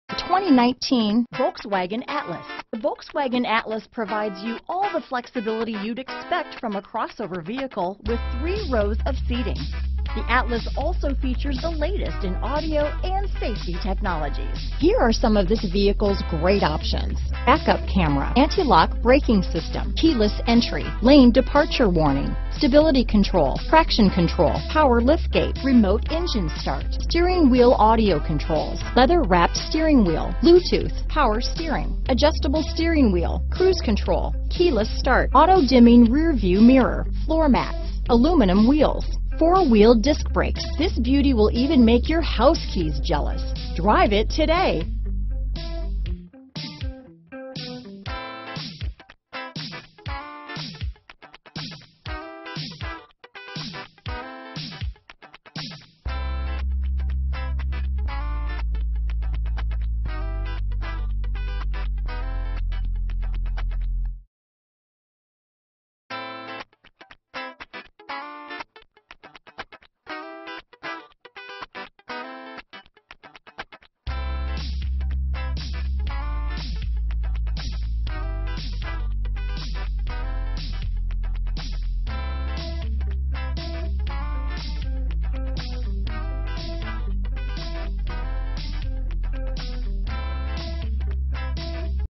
2019 Volkswagen Atlas Orlando, Sanford, Kissimme, Clermont, Winter Park, FL 90690
Pure White New 2019 Volkswagen Atlas available in Orlando, Florida at Dave Maus VW North. Servicing the Sanford, Kissimme, Clermont, Winter Park, FL area.
2019 Volkswagen Atlas 2.0T SE w/Technology - Stock#: 90690 - VIN#: 1V2WP2CA6KC570882

White 2019 Volkswagen Atlas 2.0T SE w/Technology FWD 8-Speed Automatic with Tiptronic 2.0L TSIRecent Arrival! 22/26 City/Highway MPG